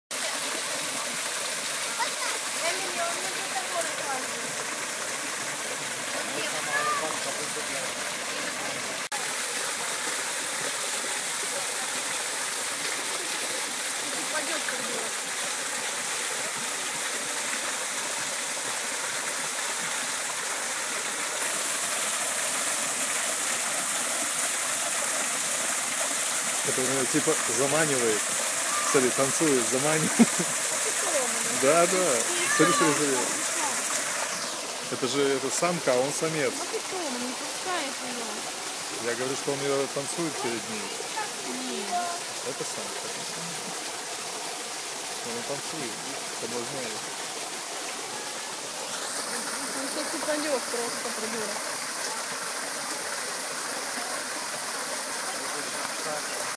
Noble peacock in lovely dance (HD)
Funniest videos Very lovely peacock with feathers open wooing before modest female peacock